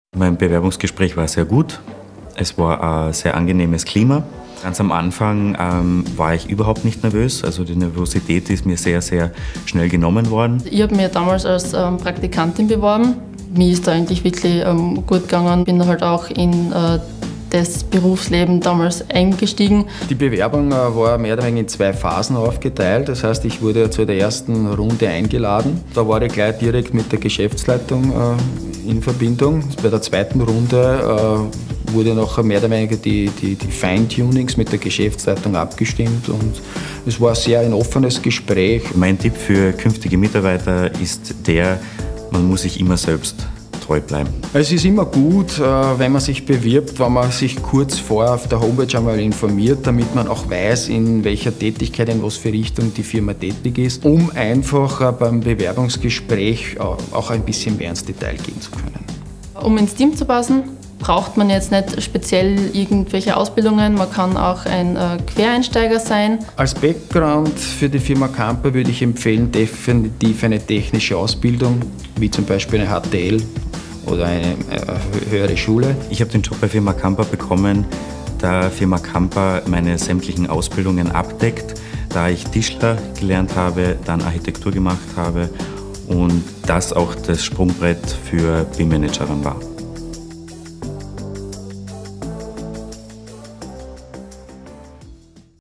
Bewerbung bei Kamper | karriere.at
Mitarbeiter erzählen, wie sie beim Bewerbungsgespräch bei Kamper punkten konnten.

Das Bewerbungsgespräch wird in einer angenehmen Atmosphäre geführt. Praktikanten haben immer wieder die Möglichkeit, im Unternehmen zu bleiben. Das Bewerbungsverfahren läuft in mehreren Runden ab.

Der karriere.reporter von karriere.at bei Kamper:
Interview mit
Ing. Johannes Schatzer Projektleitung
Sebastian Pakosta, BSc BIM Manager
Alexandra Lukas  Sachbearbeitung TGA